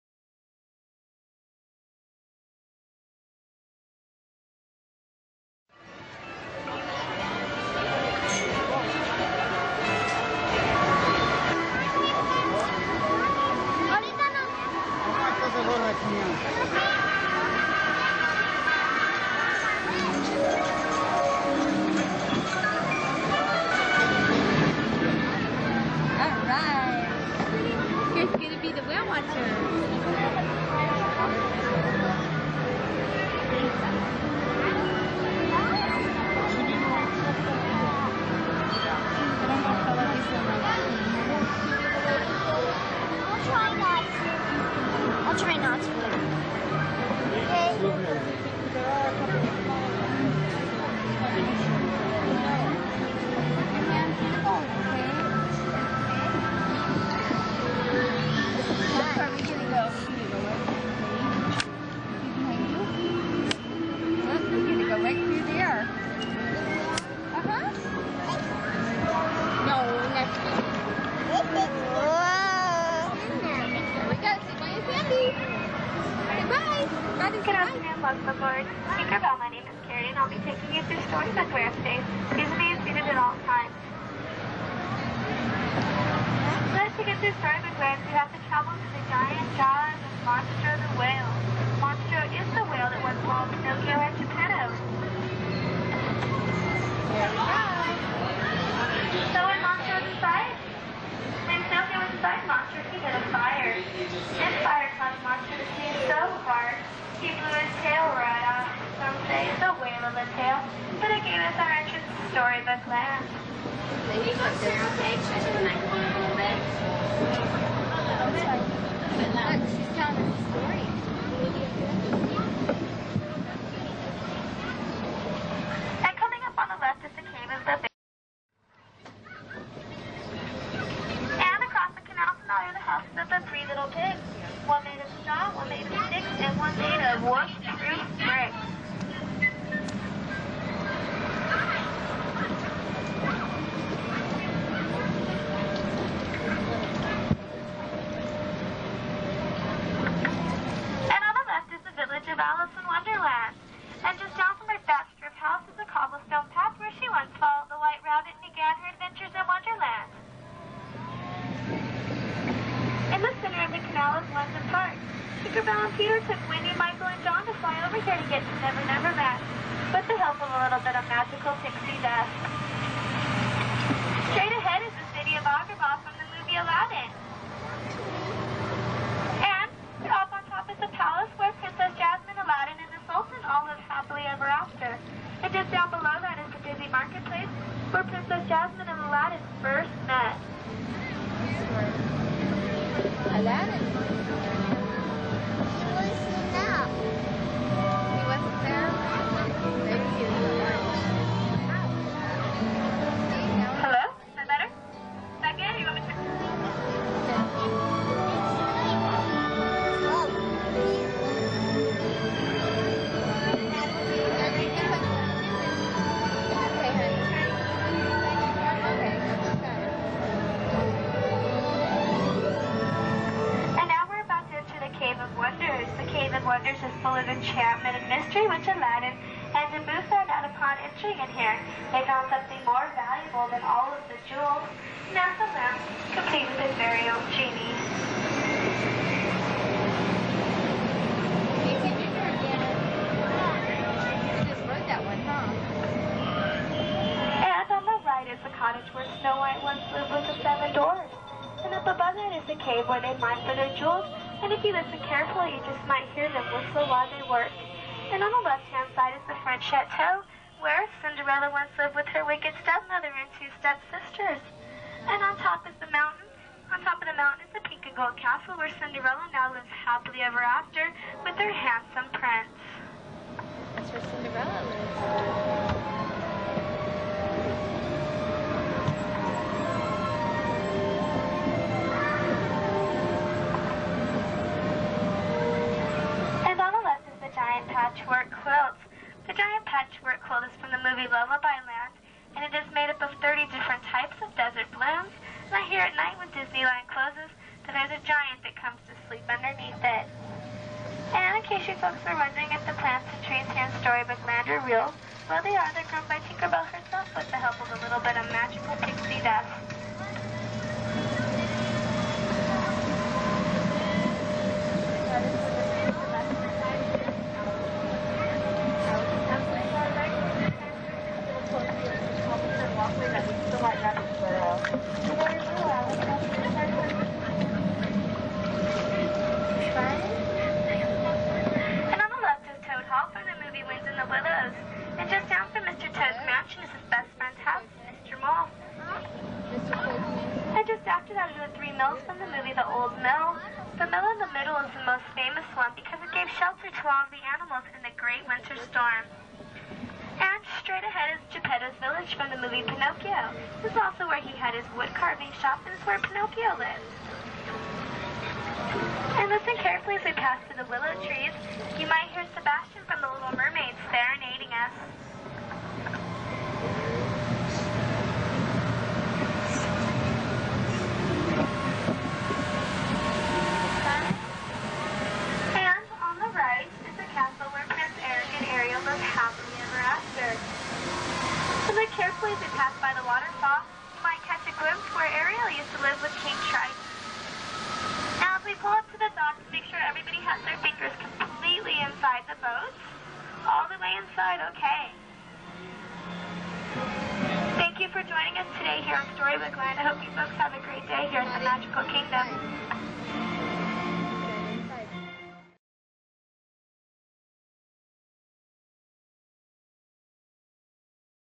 Disneyland's Storybook Land Canal Boats 1997
1997  Storybook Land Canal Boats ride shows the different stories of Disney in miniature along the canal.